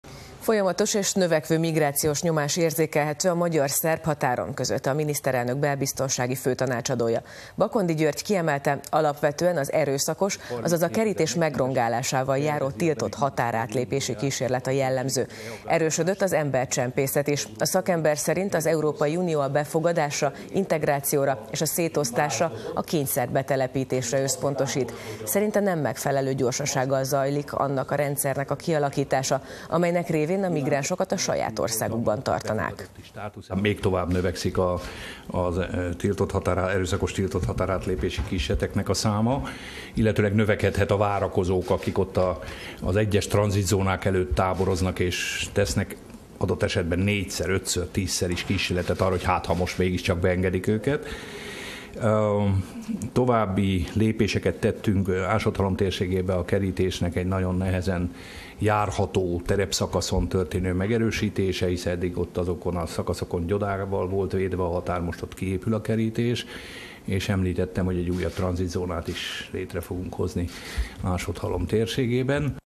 Migrációs nyomás a határon - Echo Tv
Folyamatos és növekvő migrációs nyomás érzékelhető a magyar-szerb határon - közölte a miniszterelnök belbiztonsági főtanácsadója. Bakondi György kiemelte: alapvetően az erőszakos - azaz a kerítés megrongálásával járó - tiltott határátlépési kísérlet a jellemző. Erősödött az embercsempészet is. A szakember szerint az Európai Unió a befogadásra, integrációra és a szétosztásra, a kényszerbetelepítésre összpontosít. Szerinte nem megfelelő gyorsasággal zajlik annak a rendszernek a kialakítása, amelynek révén a migránsokat a saját országukban tartanák.